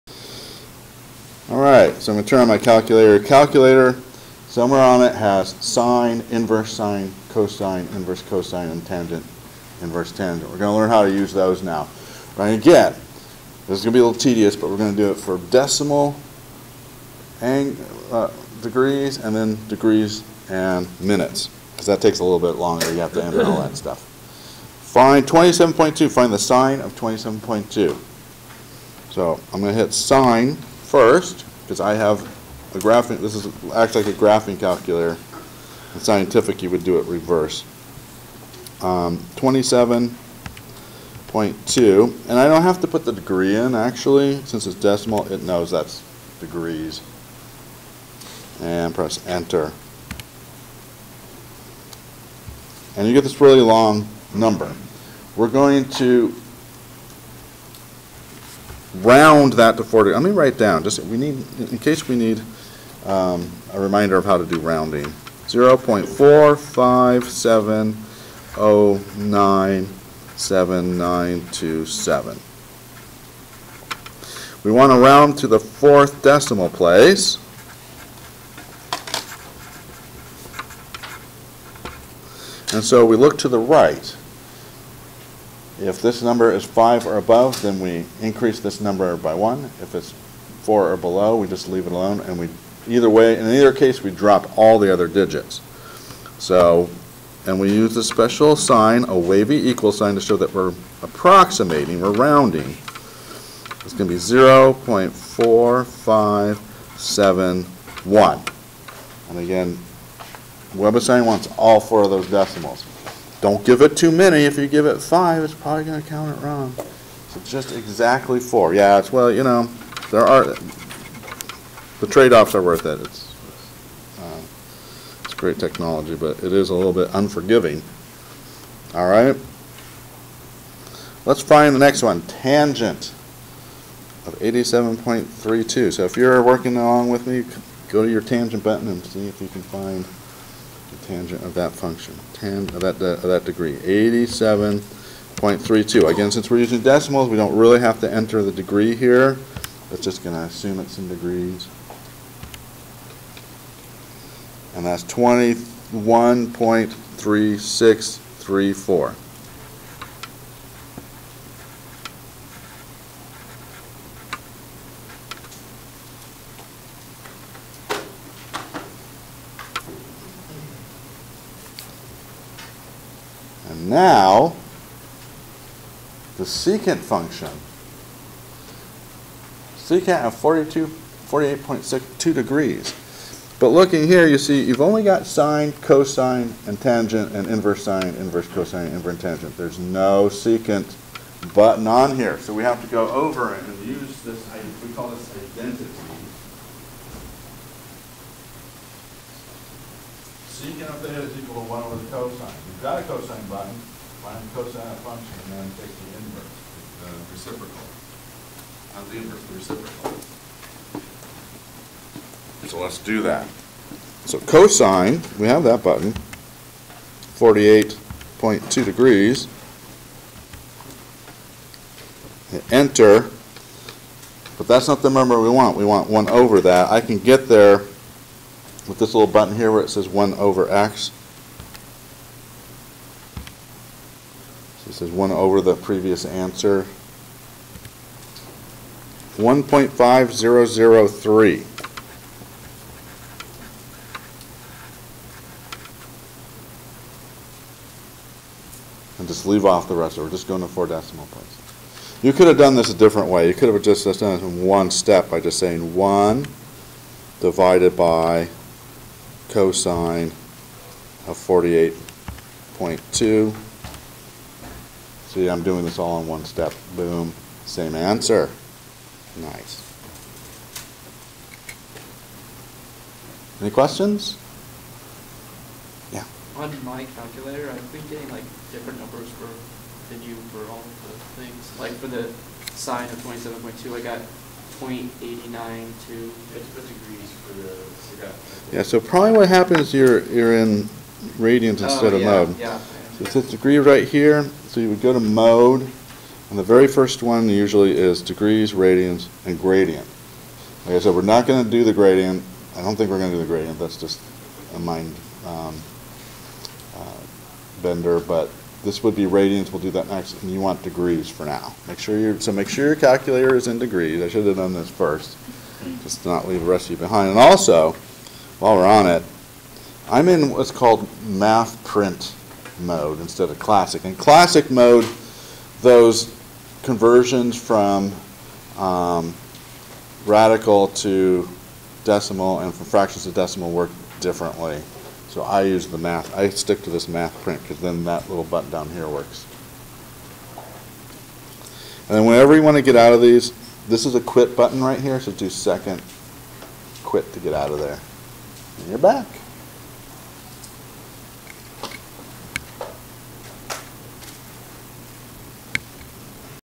TI-36X Pro Basic Trigonometric Functions Using Decimal
Find sin, tan and sec in decimal degree format